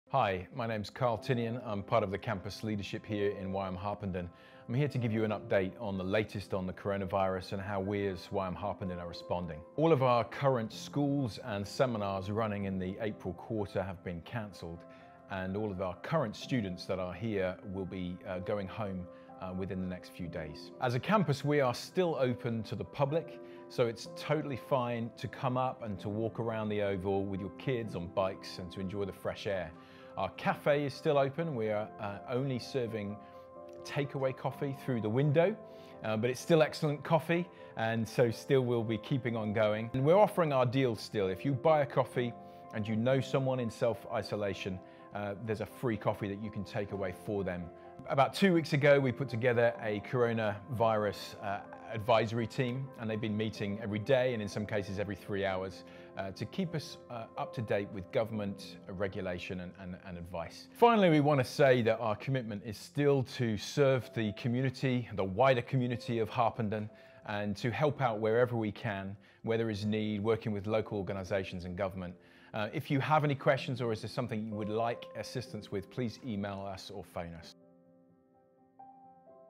YWAM Harpenden Covid-19 I UPDATE 2
COVID-19 Update 2

MESSAGE FROM THE CAMPUS LEADERSHIP REGARDING OUR UPCOMING
TRAINING PROGRAMS – MARCH 17, 2020

YWAM Harpenden like many others around the world, are working to respond to the current situation.

*PLEASE NOTE that the free coffees are only for seniors and single parents who are in isolation.